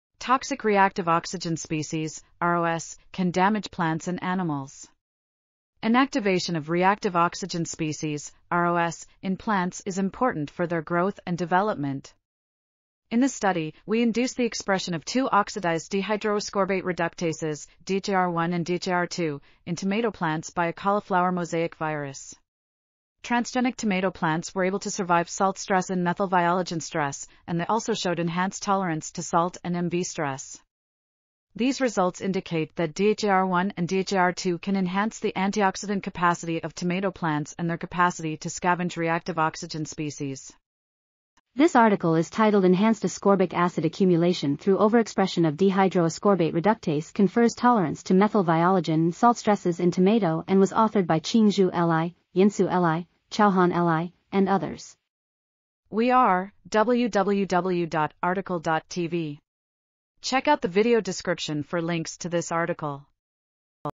Enhanced ascorbic acid accumulation through overexpression of dehydroascorbate reduct... | RTCL.TV
### Keywords ###

### Article Details ###
Title: Enhanced ascorbic acid accumulation through overexpression of dehydroascorbate reductase confers tolerance to methyl viologen and salt stresses in tomato
Authors: Qingzhu LI, Yansu LI, Chaohan LI ,and Xianchang YU
Publisher: Czech Academy of Agricultural Sciences
DOI: 10.17221/100/2011-CJGPB

### Video Timestamps ###
0:00:00 - Summary
0:00:41 - Title
0:00:55 - Outro
0:01:03 - End